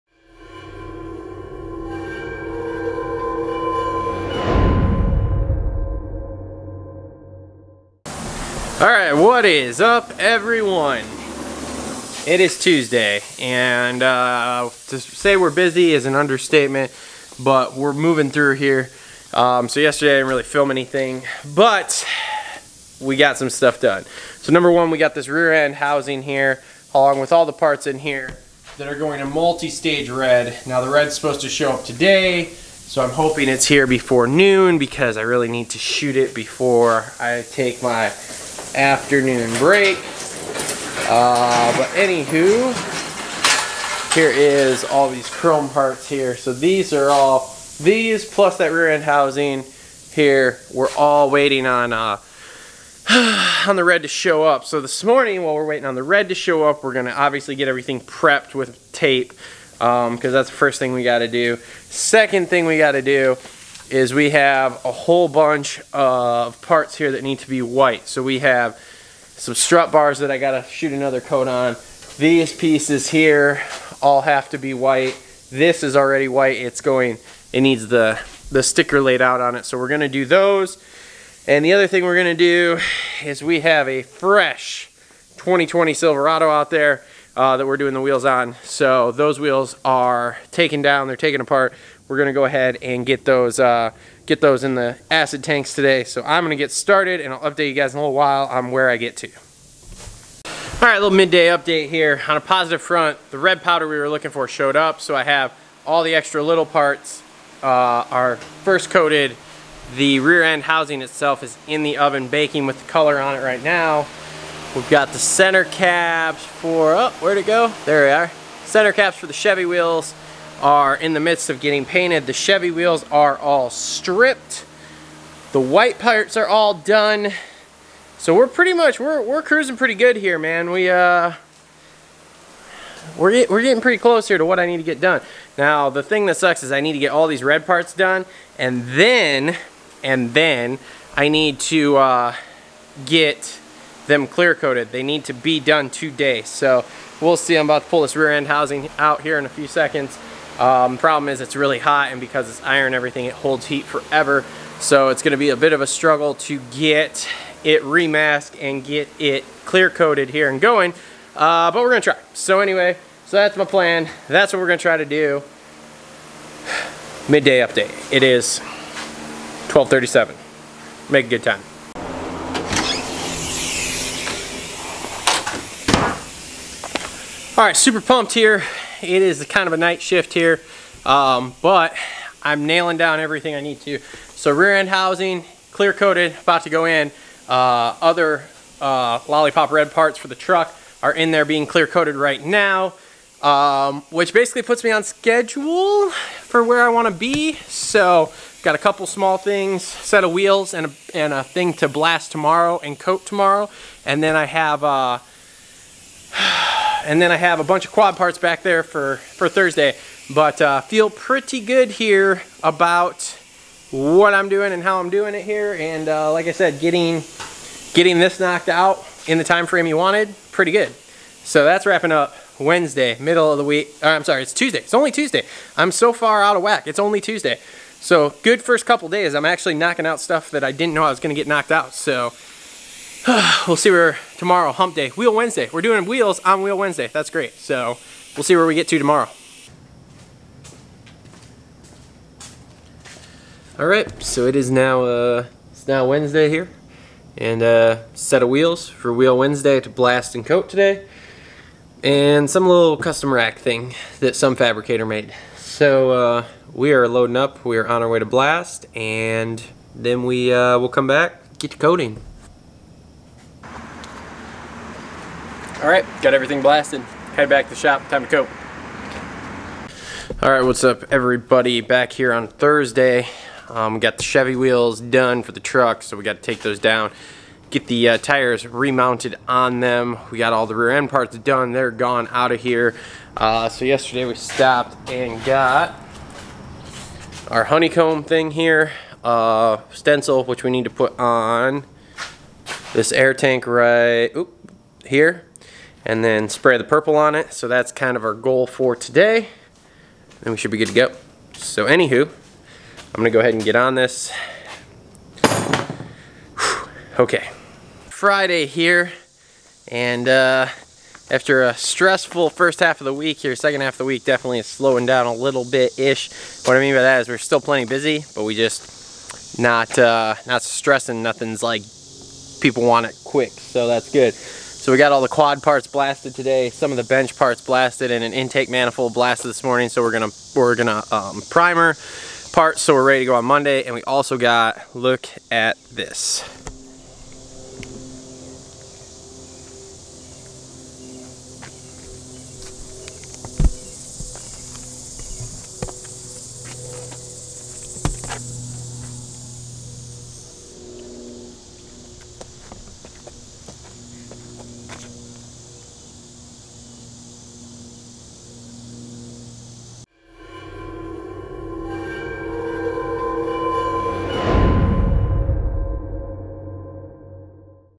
Powder Coating 2020 Silverado Wheels l TR Vlogs S3 W31
On this episode of TR Vlogs S3 W31, we refinishe the wheels on a brand new 2020 Chevy Silverado. This customer called the shop when he initially special ordered the truck telling us that even though this was a complete special order, Chevy would not sell him 18 inch black rims. Luckily for him we offer full service. So after taking delivery of the truck his first stop was to see us. We completed the job for him and created what we dubed "The Storm Trooper Edition" Chevy Silverado. What do you guys think?

Need powdercoating?
TLRCoatings.com

Camera We Use
Timelapse Camera